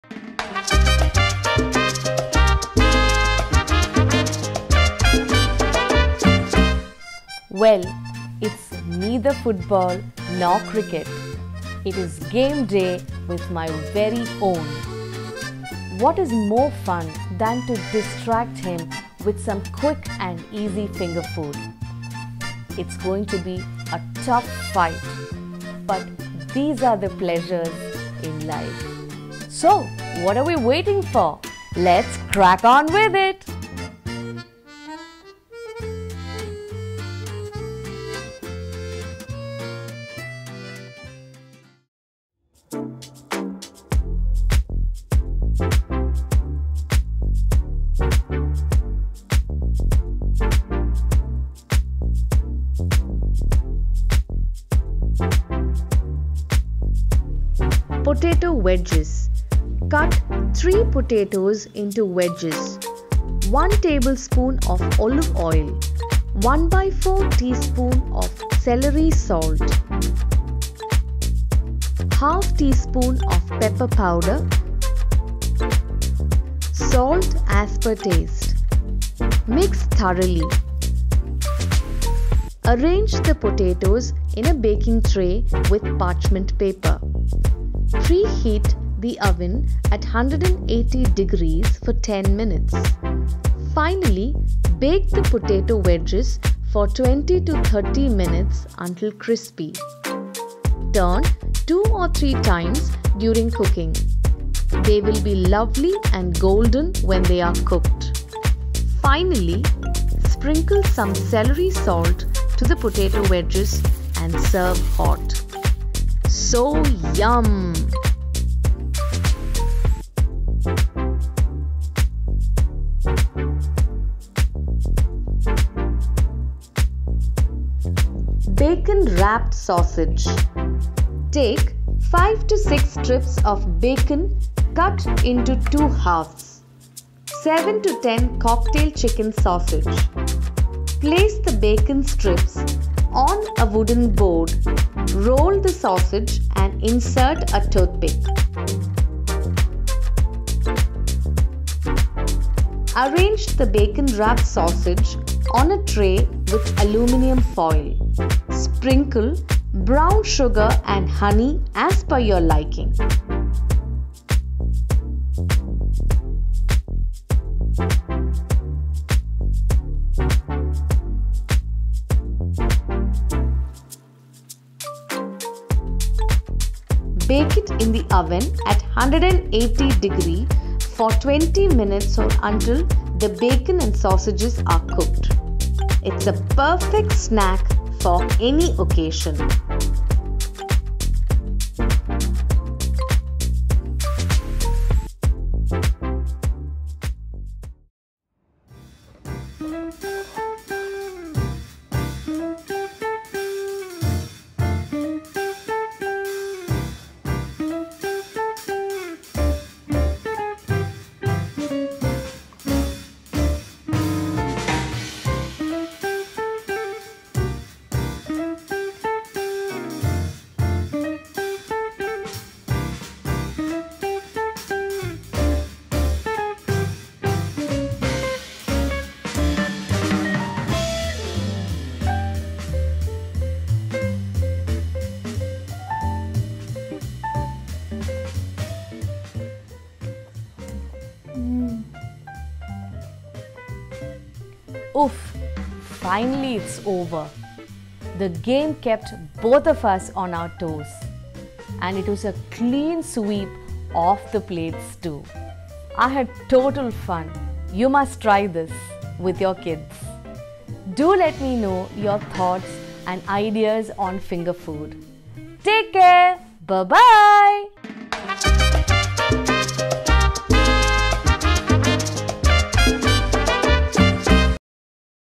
Game day snacks
Description:-
Game-day always revives my spirits and takes me down memory lane to my childhood days. Moreover, the snacks that we eat while playing is what makes the game even more enjoyable. Some simple bite-sized snacks are what puts us in higher spirits and the competition is mainly on the food. In this video, I share a couple of simple recipes which I’m sure you will enjoy - Potato wedges, and Bacon-wrapped sausages. The savoury dish of Potato wedges can even be used as a party snack and is guaranteed to put a blissful expression on your face. The delicious, juicy Bacon-wrapped sausages will help put your thinking caps on in the middle of any game. Game-day

Ingredients:-

Potato cut into wedges - 3
Olive oil - 1 tbsp
Celery salt - ¼ tsp
Preheat the oven to 180 degrees for 10 minutes

Bake wedges for 20-30 minutes until crispy, keep turning the potatoes 2-3 times while it’s cooking. Sprinkle some Celery salt.

Bacon-wrapped_sausages

Bacon - 5-6 strips [cut into half]
Chicken sausages - 7-10
Brown sugar - As per taste
Honey - As per taste

Preheat the oven to 180 degrees for 10 minutes. Bake the Bacon-wrapped sausages for 20 minutes until it’s golden-brown and cooked

Credits:
Editor - Vignesh